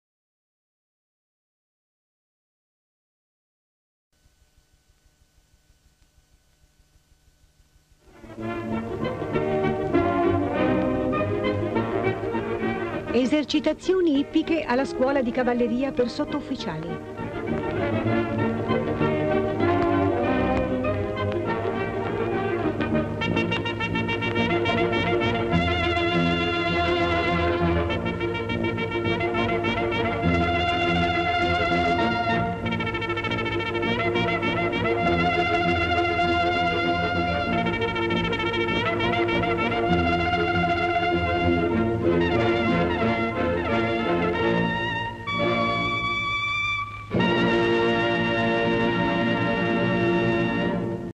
Esercitazioni ippiche alla scuola di cavalleria per sottoufficiali.
Descrizione sequenze:allievi della scuola sottoufficiali a cavallo schierati prima dell'esercitazione ippica ; i cavalli ; inizia l'esercitazione ippica, gli allievi eseguono esercizi di trotto ; salto degli ostacoli ;